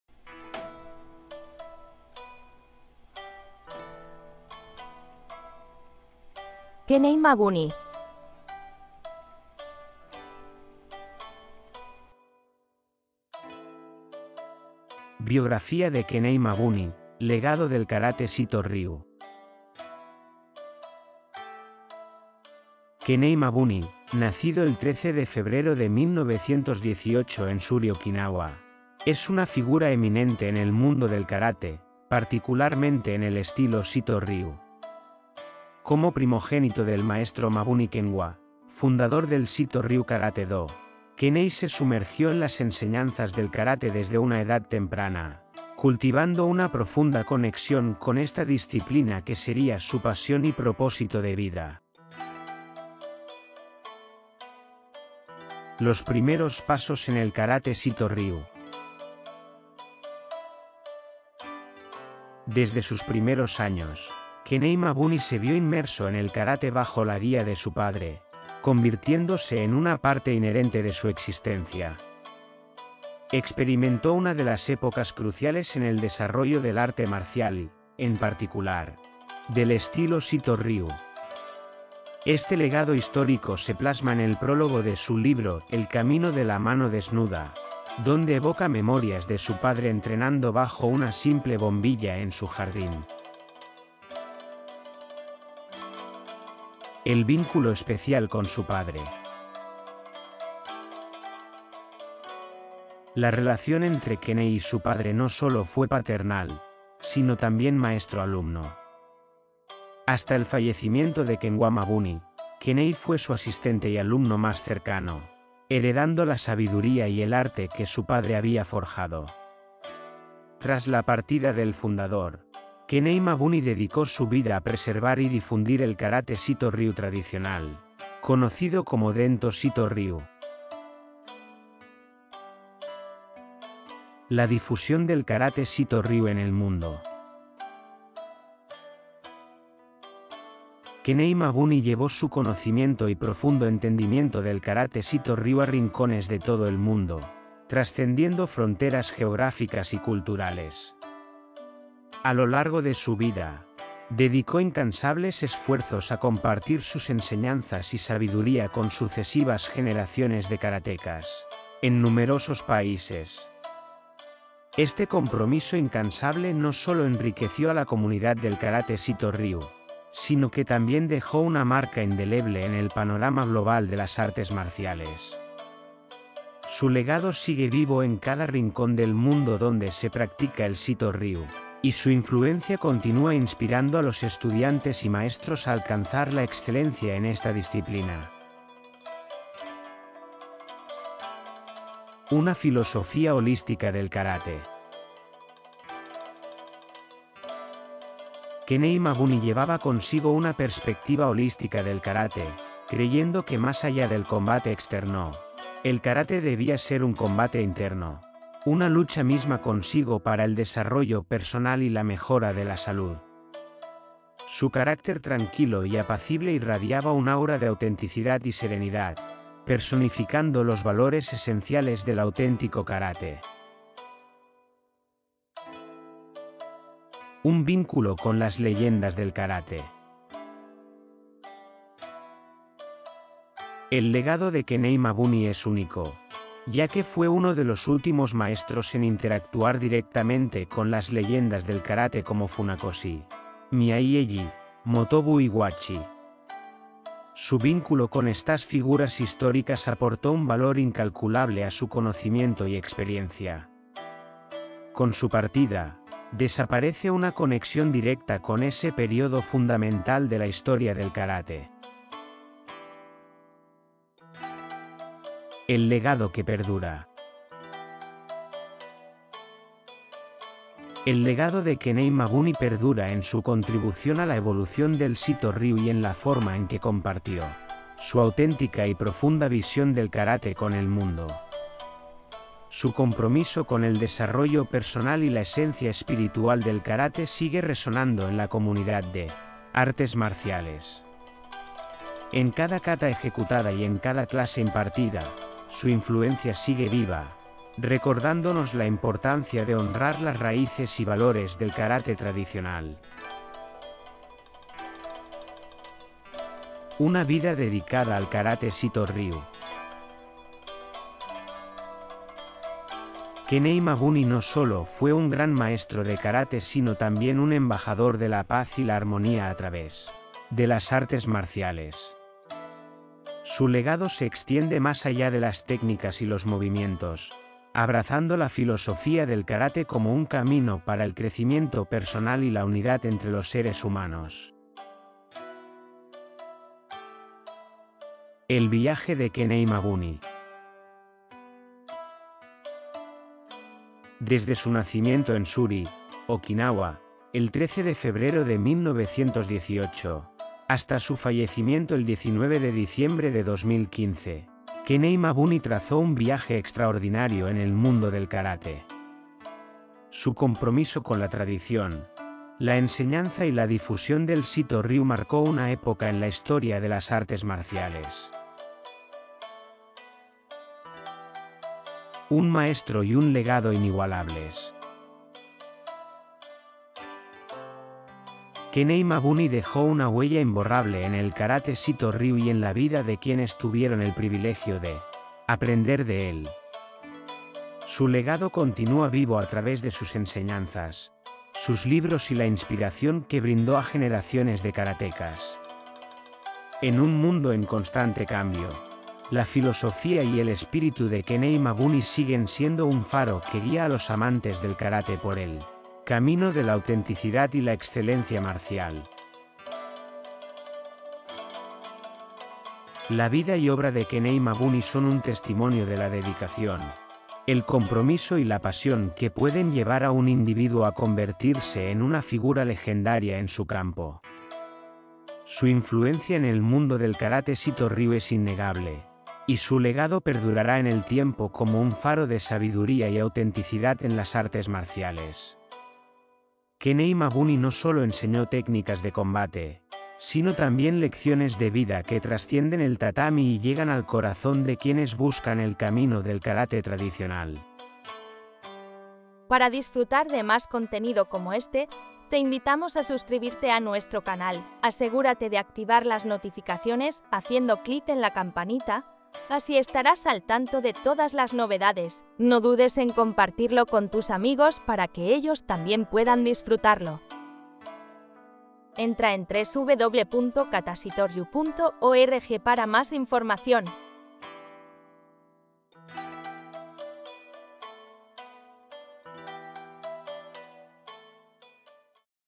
Kenei Mabuni, Biografía katashitoryu.org
Kenei Mabuni, nacido el 13 de febrero de 1918 en Shuri Okinawa, es una figura destacada en el mundo del karate, especialmente en el estilo Shito-Ryu. Como hijo del Maestro Kenwa Mabuni, fundador del Shito-Ryu Karate-Do, Kenei se sumergió en las enseñanzas del karate desde temprana edad, desarrollando una profunda conexión con esta disciplina que se convertiría en su pasión y propósito de vida.

Desde sus primeros años, Kenei estuvo inmerso en el karate bajo la guía de su padre, convirtiéndose en una parte integral de su vida. Después de la muerte de su padre, Kenei se dedicó a preservar y difundir el Karate Shito-Ryu tradicional, conocido como Dento Shito-Ryu, llevando sus conocimientos a nivel mundial y dejando una huella imborrable en la comunidad del karate Shito-Ryu.

Kenei Mabuni compartía una filosofía holística del karate, creyendo en su poder para el desarrollo personal y la mejora de la salud más allá del combate físico. Su influencia perdura en la comunidad de artes marciales y su legado sigue vivo en cada rincón del mundo donde se practica el Shito-Ryu. Kenei tuvo el honor de interactuar con leyendas del karate como Funakoshi, Miyagi, Motobu y Uechi, aportando un valor incalculable a su conocimiento y experiencia.

Su vida y obra son un testimonio de dedicación, compromiso y pasión en el campo del karate Shito-Ryu, y su legado continúa siendo una fuente de inspiración para quienes buscan el camino del karate tradicional y la excelencia marcial.